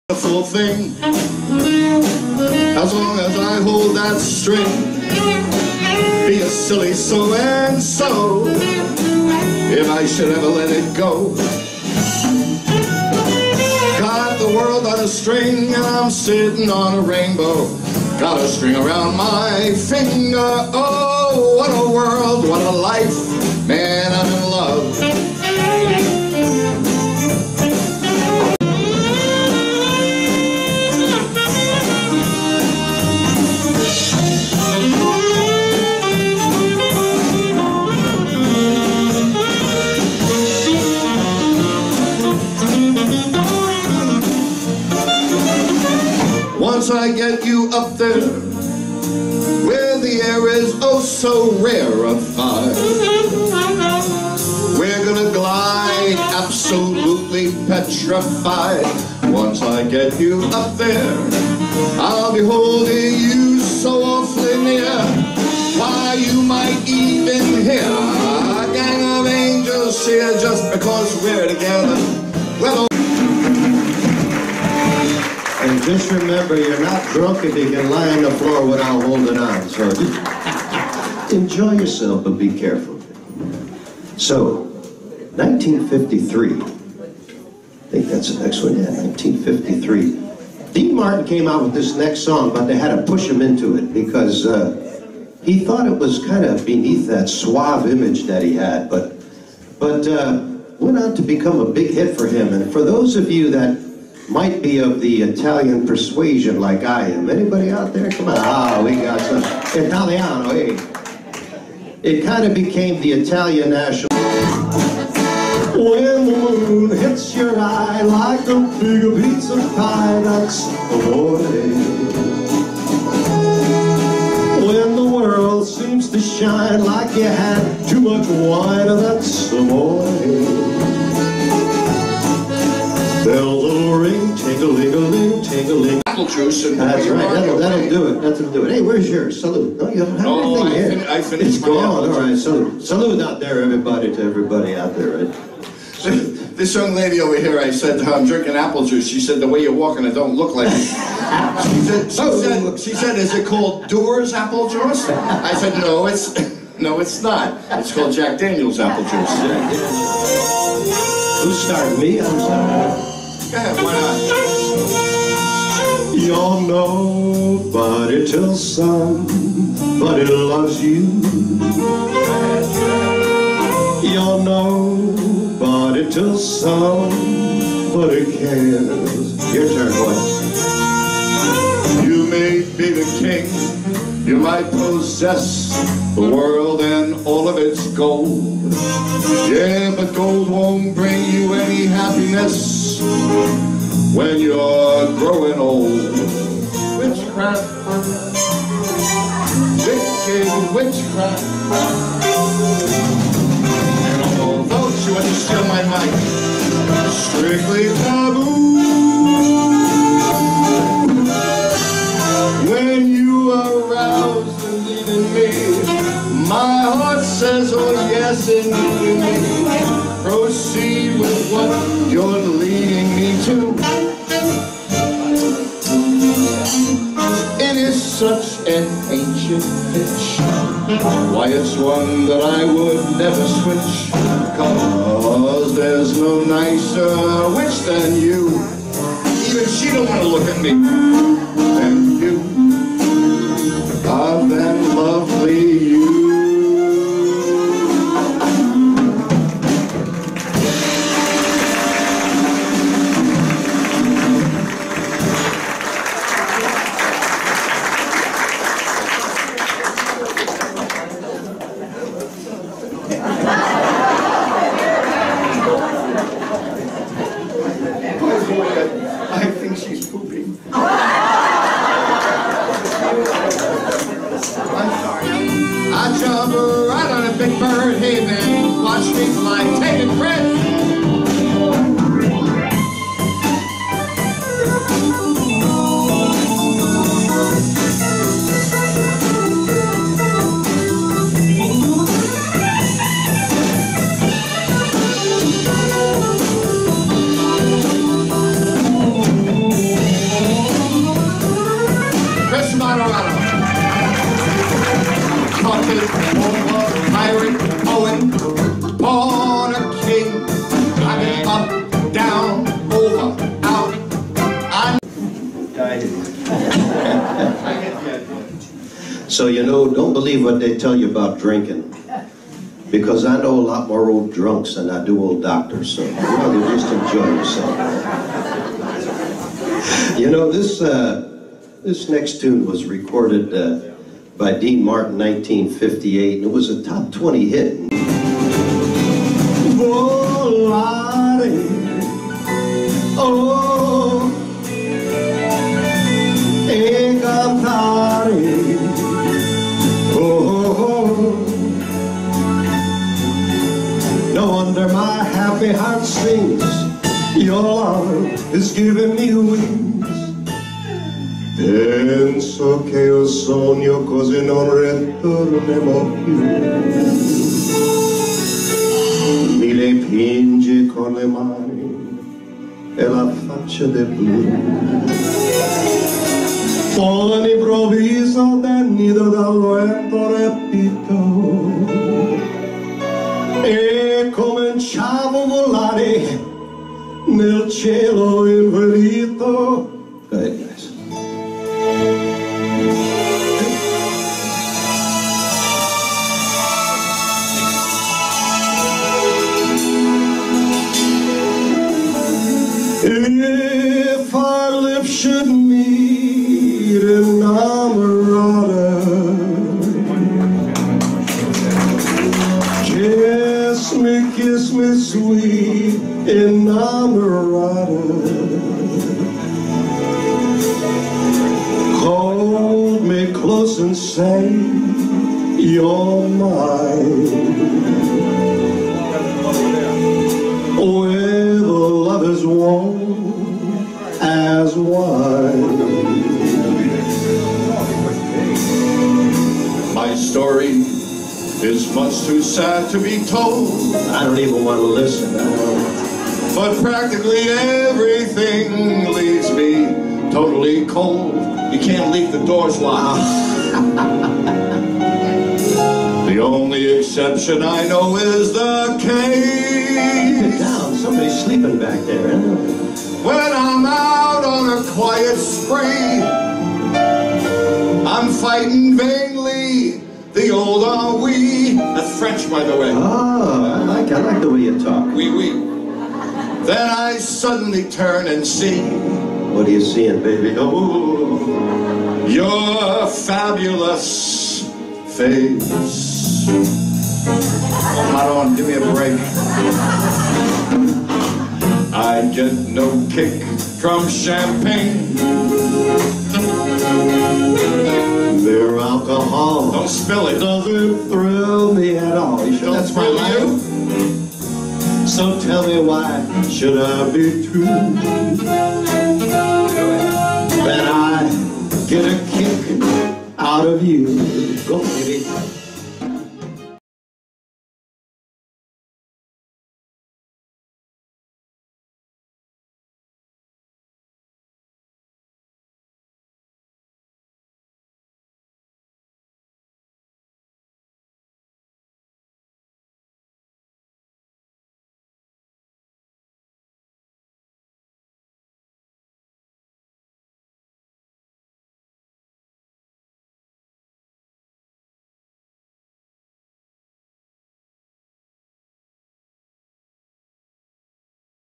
Arts on the Lake10 min
For all are interested in seeing a 10 minute segment of "The Arts On The Lake" Performance of Frank and Dean...A Tribute.